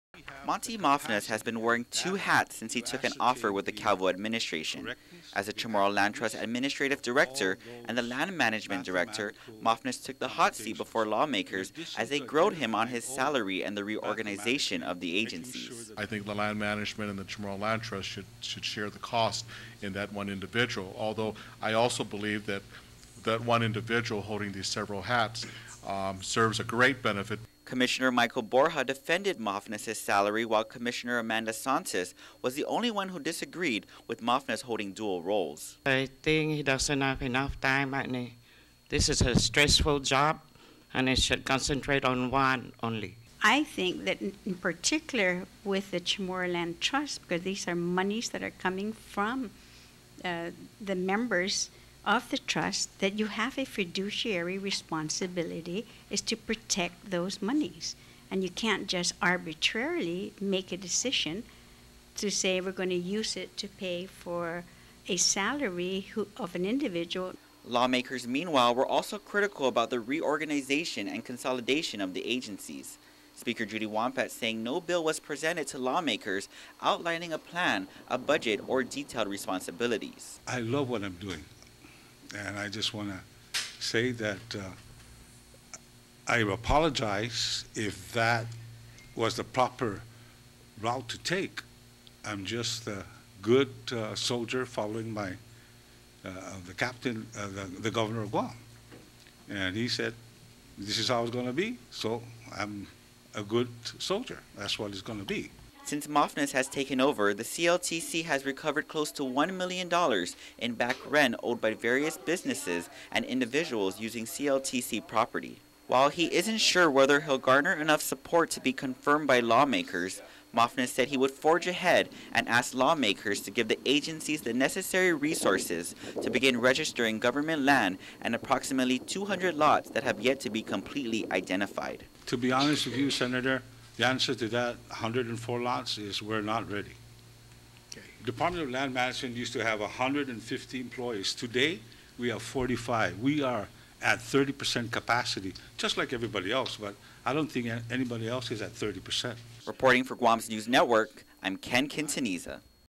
Monte Mafnas takes the hot seat
It's been five months since Governor Eddie Calvo consolidated the Chamorro Land Trust Commission and Ancestral Lands Commission with the Department of Land Management.